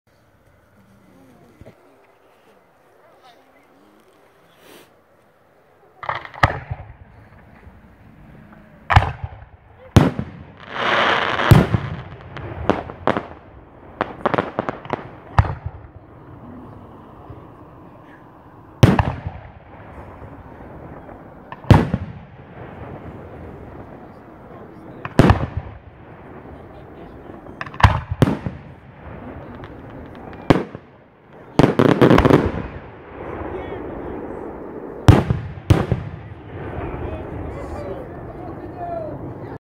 8x 4 inch (100mm) shells
8x Mixed 4-inch shells.

7x Normal
1x Peanut